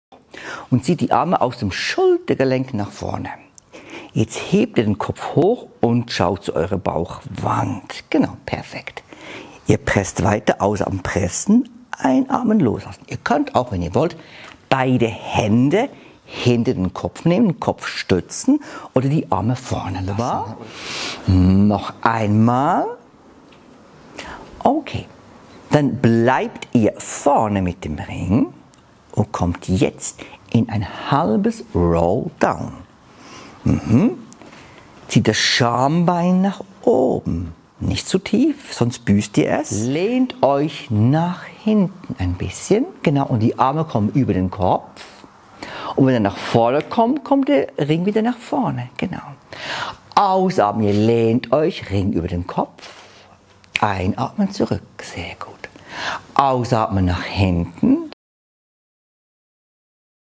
Magic Circle 1 Basic mit Andy Raaflaub
Das Training mit dem Magic Circle ist cool und effektiv. In dieser Session lernst du ganz viele Basic Übungen kennen. Dies ist ein 3-teilige aufbauendes Set. Bereite dich vor für den nächsten Schritt: „Magic Circle Intermediate“.
Level: 1 — Trainer: Andy Raaflaub
Gerät: Matte, Magic Circle   — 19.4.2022